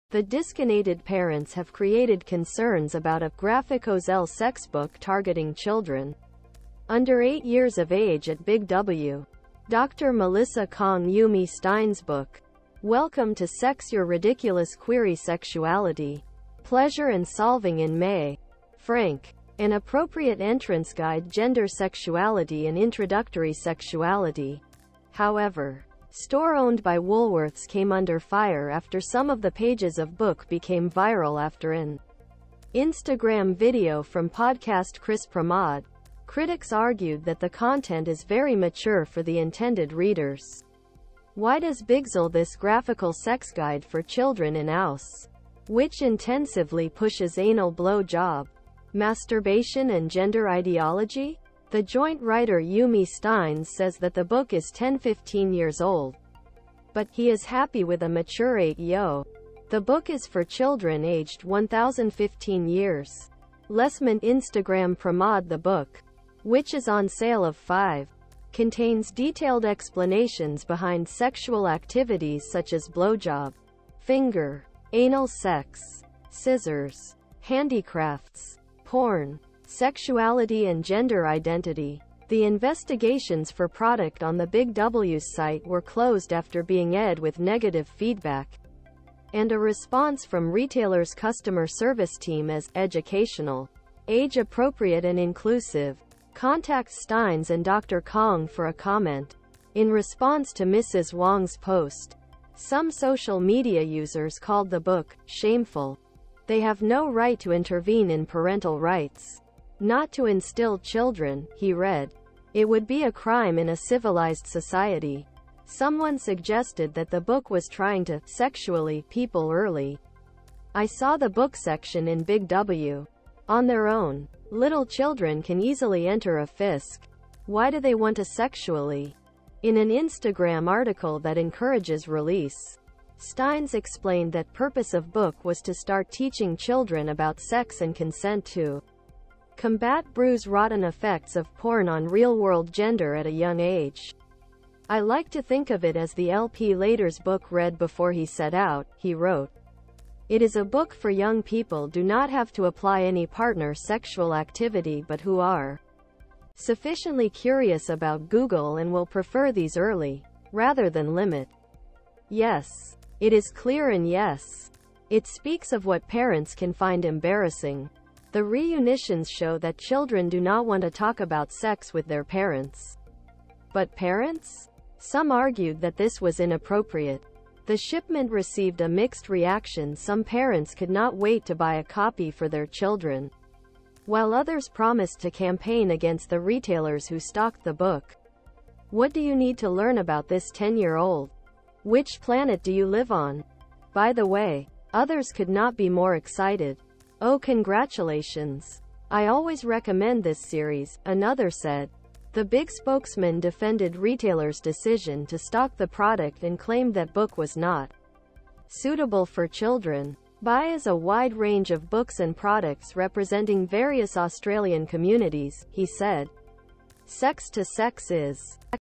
Yumi Stynes Sex for Children in BigSparks Discussion
The disconated parents have created concerns about a “graphic özel sex book targeting children under eight years of age at Big W.  Dr Melissa Kang Yumi Stynes' book, Welcome to Sex Your ridiculous query sexuality, pleasure and solving in May, “Frank, an appropriate entrance guide gender sexuality an introductory sexuality”. However, store owned by Woolworths came under fire after some of the pages of book became viral after an Instagram video from Podcast Chris Primod. Critics argued that the content is very mature for the intended readers. “Why does Bigsell this graphical sex guide for children in Aus, which intensively pushes analblowjob, masturbation and gender ideology? “The joint writer [Yumi Stynes] says that the book is 10-15 years old, but“ he is happy with a mature 8-yo ”.” The book is for children aged 10-15 years. Lessment InstagramPrimod The book, which is on sale of $ 5, contains detailed explanations behind sexual activities such as blowjob, finger, anal sex, scissors, handicrafts, porn, sexuality and gender identity. The investigations for product on the Big W's site were closed after being ed with negative feedback and a response from retailer's customer service team as “educational, age -appropriate and inclusive”. Contact Stynes and Dr Kang for a comment. In response to Mrs. Wong's post, some social media users called the book “Shameful”. “They have no right to intervene in parental rights, not to instill children,” he read. “It would be a crime in a civilized society. Someone suggested that the book was trying to “sexually” people early. “I saw the book section in Big W. on their own.Little children can easily 'enter a fisk'. Why do they want to sexually? In an Instagram article that encourages release, Stynes explained that purpose of book was to start teaching children about sex and consent to combat bruise rotten effects of porn on real world gender at a young age. “I like to think of it as the L-Platers book read before he set out,” he wrote. “It is a book for young people do not have to apply any partner sexual activity but who are sufficiently curious about Google and will prefer these early .rather than limit. “Yes, it is clear and yes, it speaks of what parents can find embarrassing. The reunitions show that children do not want to talk about sex with their parents. But parents? Some argued that this was inappropriate. The shipment received a mixed reaction - some parents could not wait to buy a copy for their children, while others promised to campaign against the retailers who stocked the book. “What do you need to learn about this 10 -year -old? Which planet do you live on? By the way, others could not be more excited. “Ohhh congratulations! I always recommend this series,” another said. The Bigspokesman defended retailer's decision to stock the product and claimed that book was not suitable for children. “Bighas a wide range of books and products representing various Australian communities,” he said. “Sex to sex is an educational, age-appropriate and inclusive book that contains content in accordance with the development and early experience of young people aged 12-15 years."